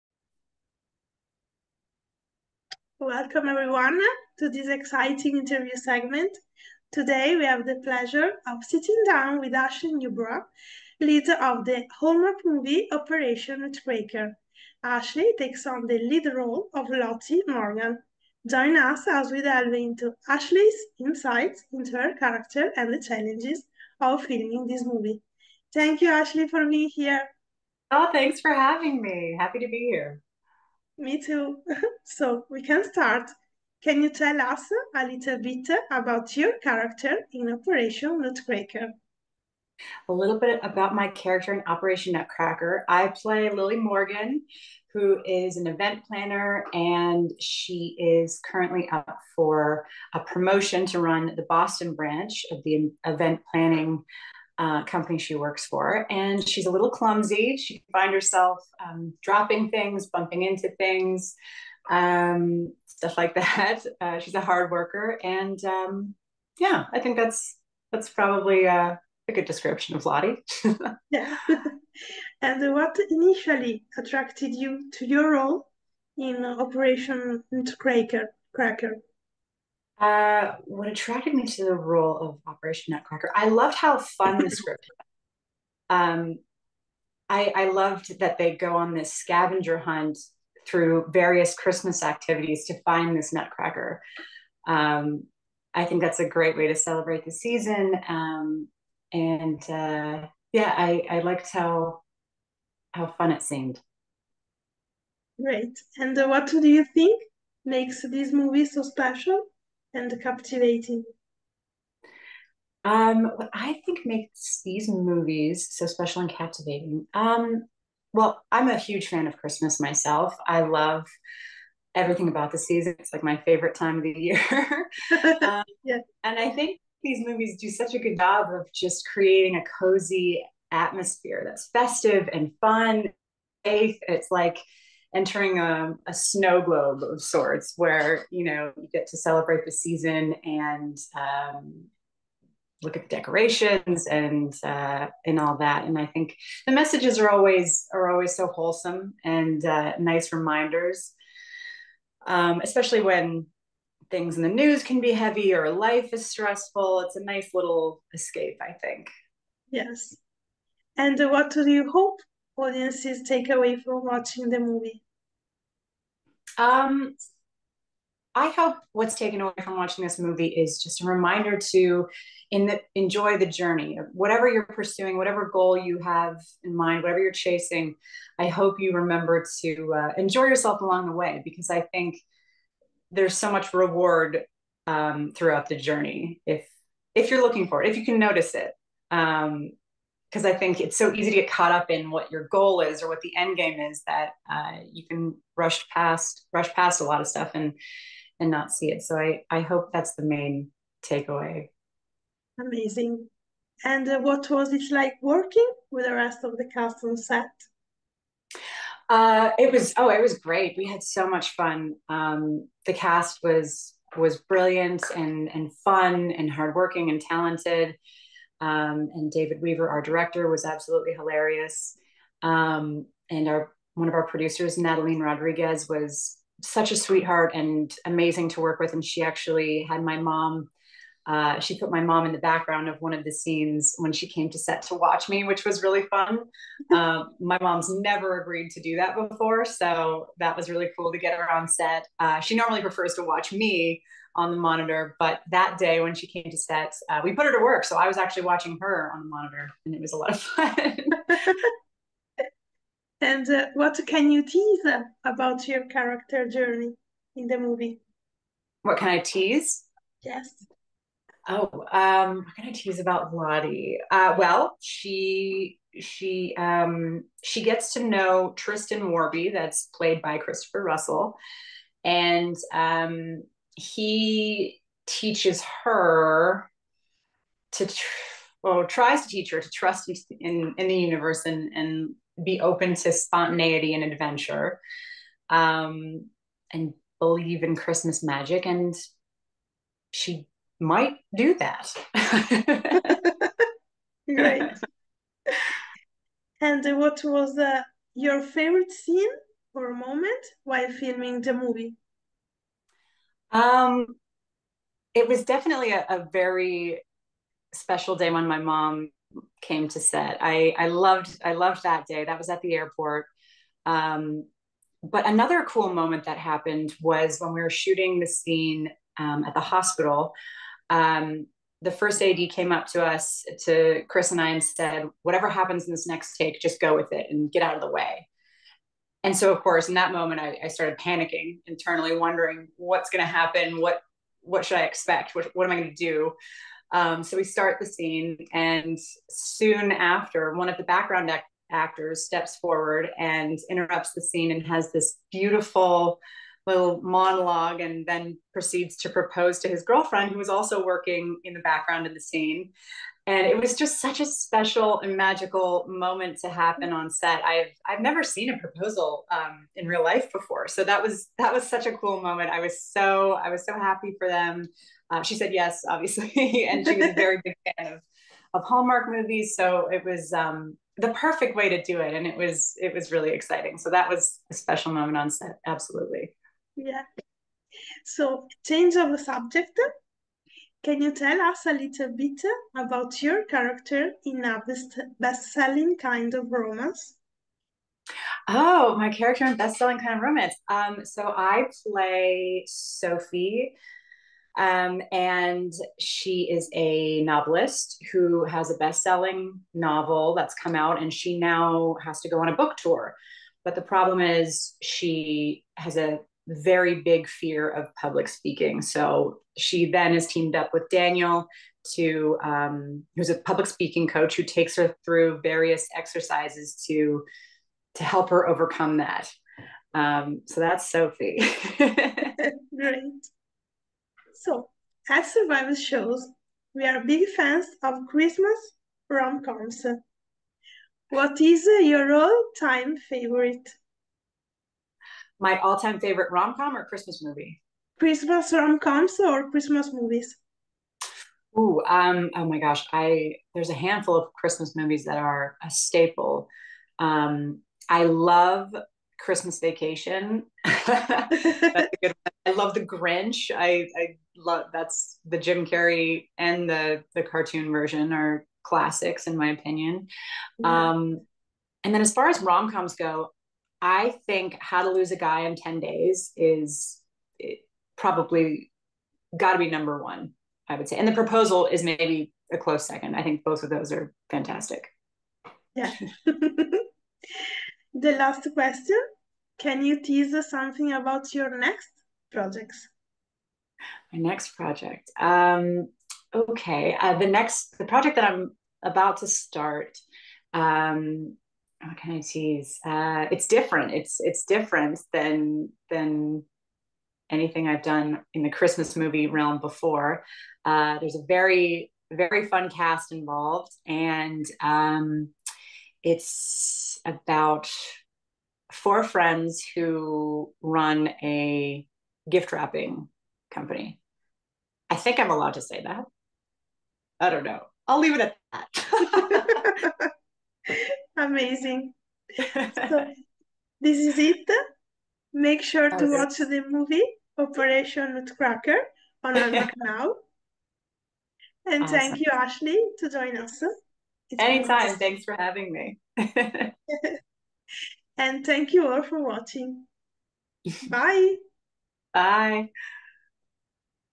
Ashley Newbrough Talks Hallmark's 'Operation Nutcracker' and Christmas Magic - EXCLUSIVE INTERVIEW
In this in-depth interview, Ashley Newbrough discusses her role in 'Operation Nutcracker' and what she hopes audiences will take away from the film. From the importance of enjoying the journey to the cozy, festive atmosphere the movie creates, Ashley's reflections are sure to warm your heart this holiday season. Catch the interview today!